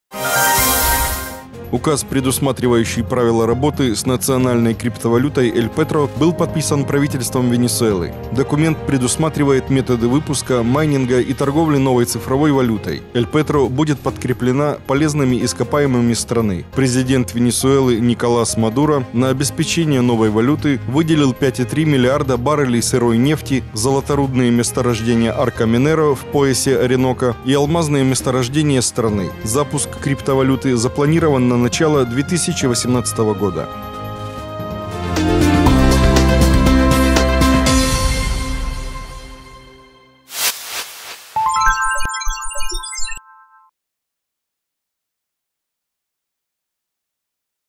Венесуэла запускает El Petro
Указ, предусматривающий правила работы с национальной криптовалютой  El Petro, был подписан Правительством Венесуэлы. Документ предусматривает методы выпуска, майнинга и торговли новой цифровой валютой. El Petro будет подкреплена полезными ископаемыми страны. Президент Венесуэлы Николас Мадуро на обеспечение новой валюты выделил 5,3 миллиарда баррелей сырой нефти, золоторудные месторождения Arco Minero в поясе Ориноко и алмазные месторождения страны. Запуск криптовалюты запланирован на начало 2018 года.